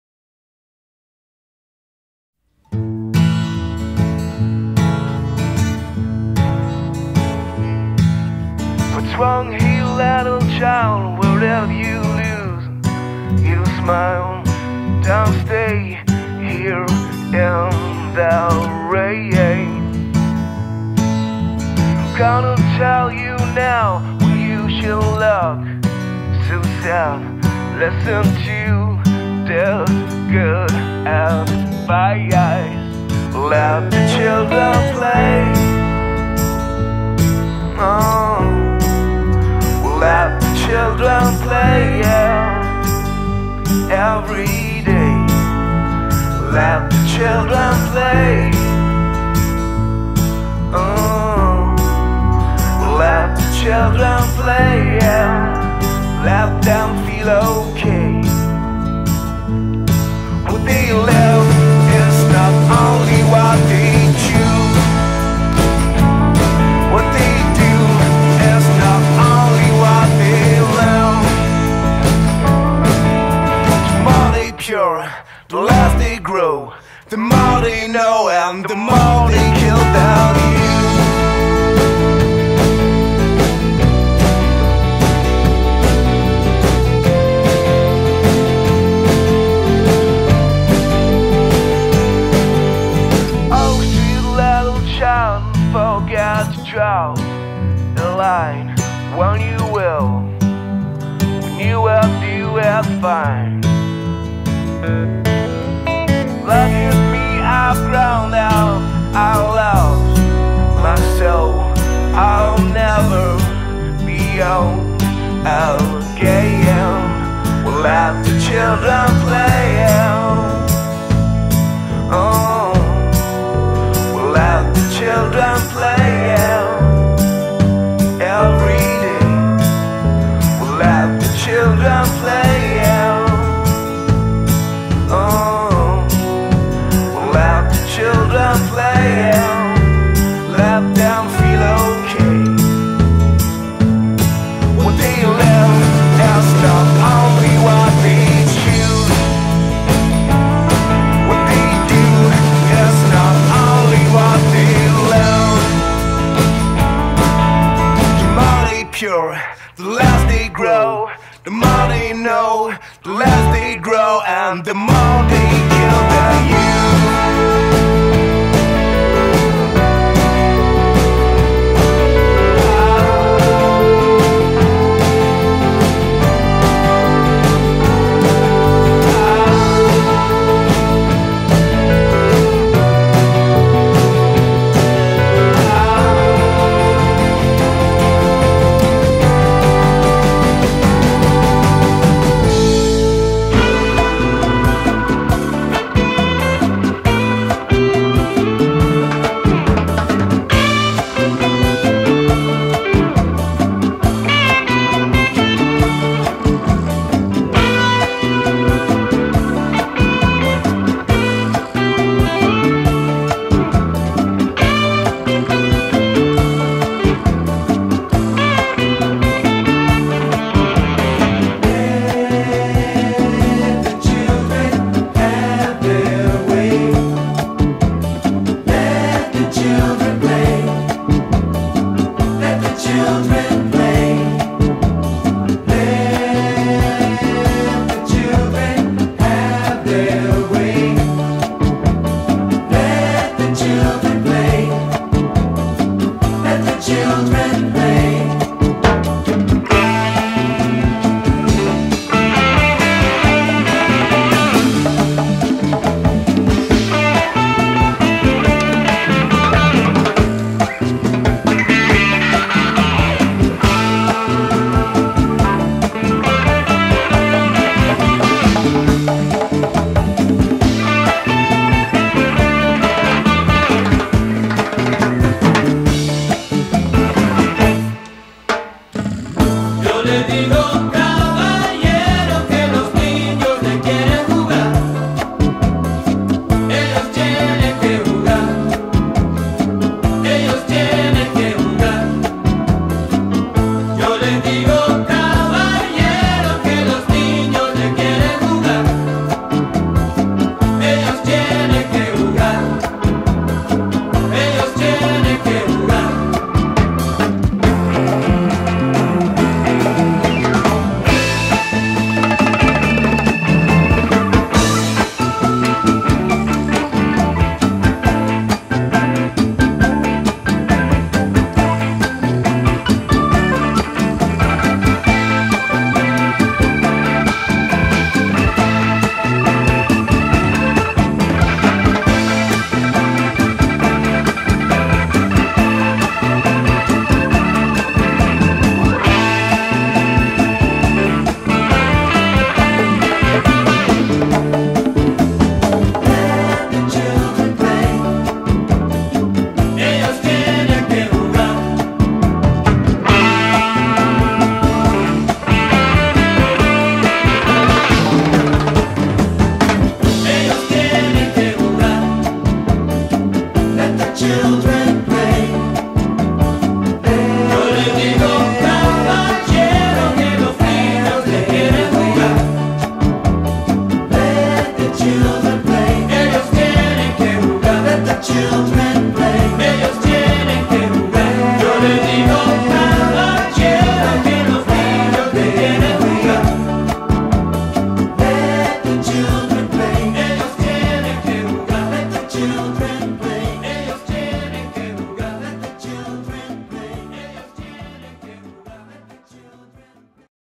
UNICEF - Let the children play
Children aound the world are being denied their rights. and give a small monthly amount to make a difference.